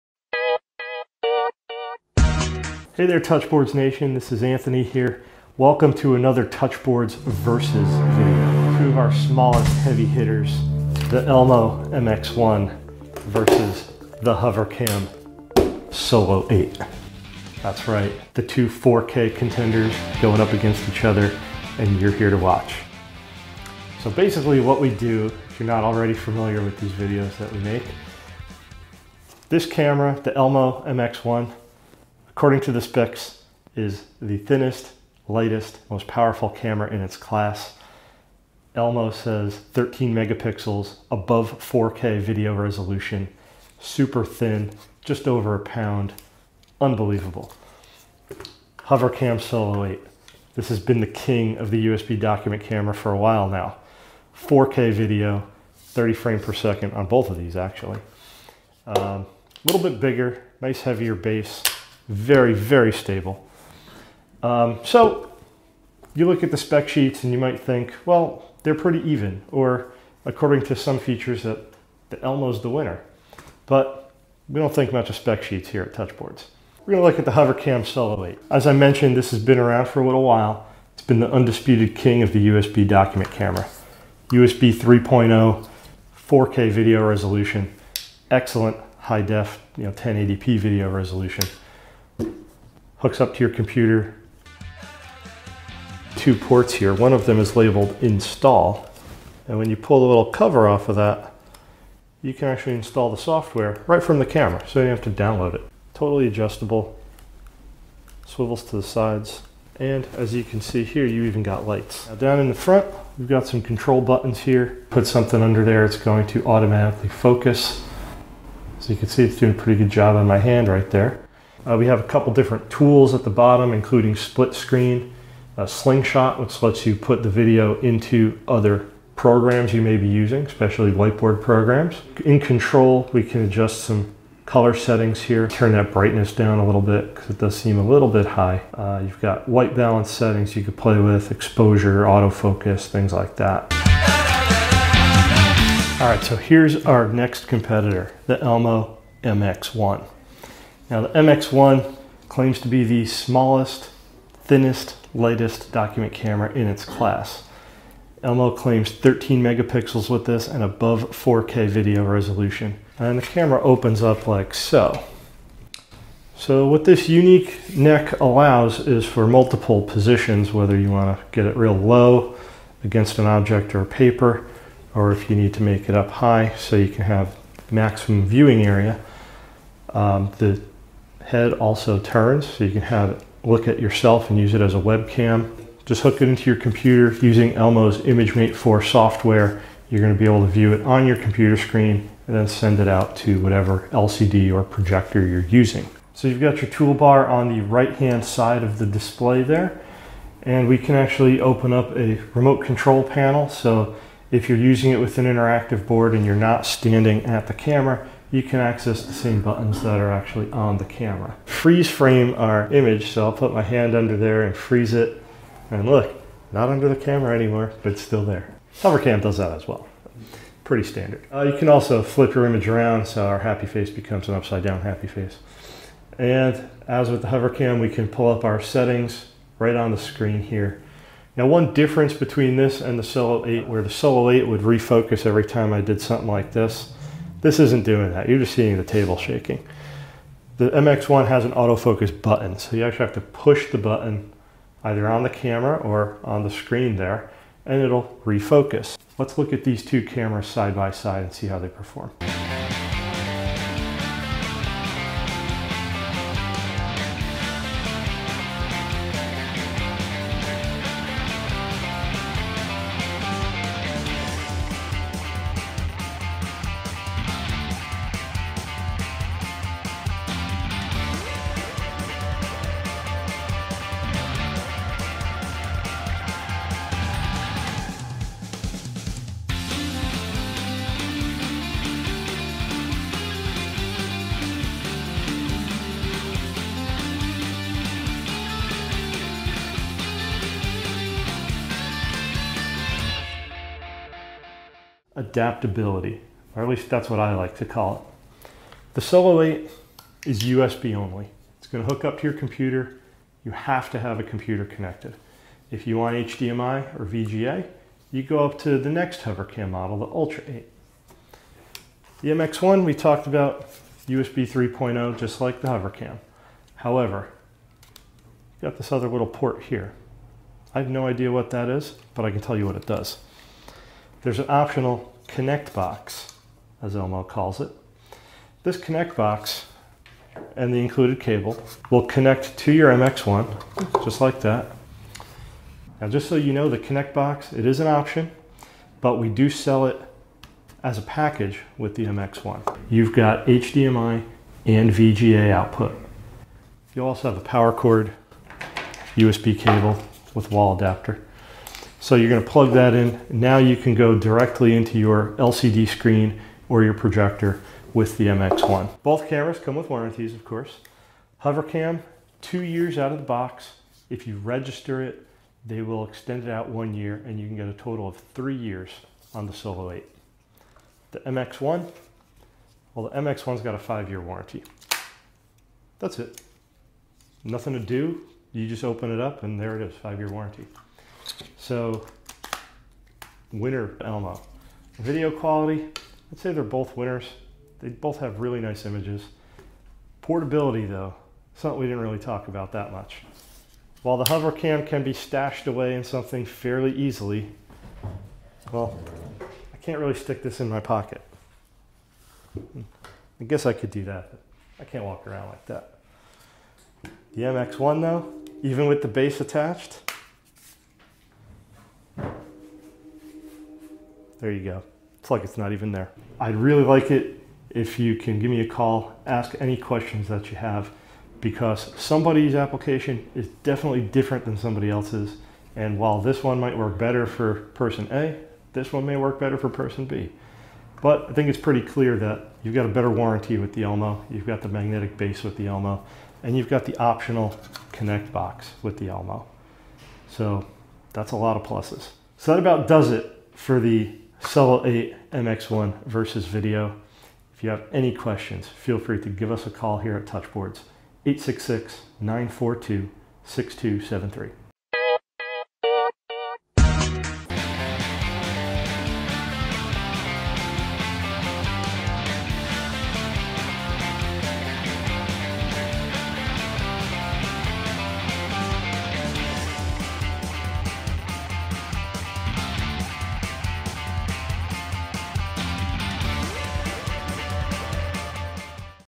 Hovercam Solo 8 Vs. Elmo MX-1 (review of each document camera side by side)
The HoverCam Solo 8 and The Elmo MX-1 are both SuperSpeed USB 3.0 document cameras capable of 4k resolution. Their spec sheets can be found all over the web, but here at Touchboards we do things a little different. We have thrown the spec sheets out and pit the two machines against each other in a performance battle. We carry a huge range of document cameras for all applications and budgets. We can help you choose the right camera.